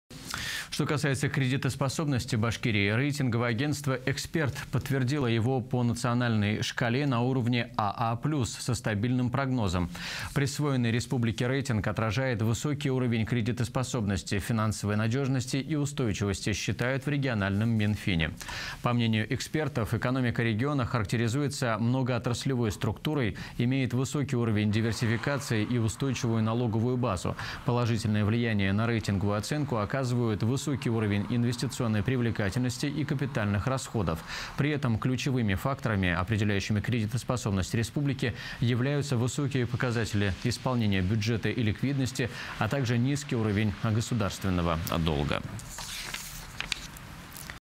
Агентство Эксперт подтвердило рейтинг РБ на уровне «АА+»
Что касается кредитоспособности Башкортостана -  рейтинговое агентство «Эксперт» подтвердило его по национальной шкале на уровне АА+, со стабильным прогнозом. Присвоенный республике рейтинг отражает высокий уровень кредитоспособности, финансовой надежности и устойчивости, считают в региональном Минфине.